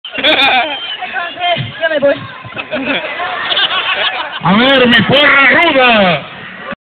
IROKA
UNA DE LAS VARIAS VESES KE A BENIDO A TUXTEPEC